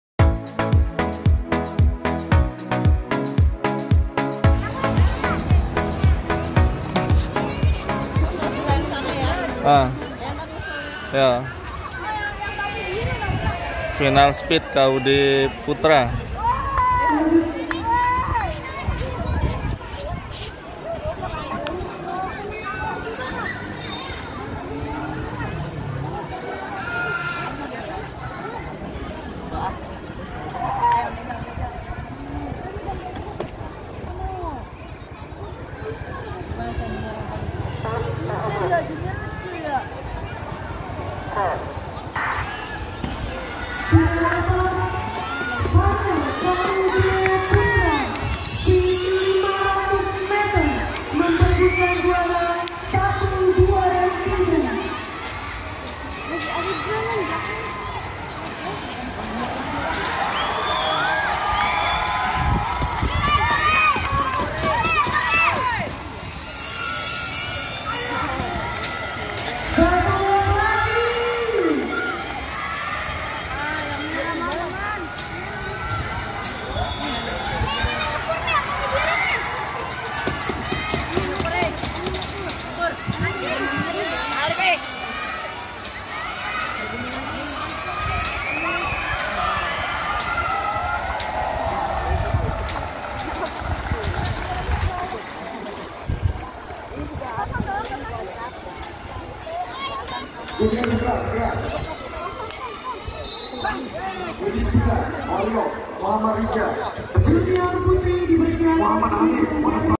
FINAL SPEED 500 METER KU-D PUTRA | GWIS OPEN 2024
Kejuaran Sepatu Roda tingkat Nasional GWIS OPEN 2024 di Kabupaten Bekasi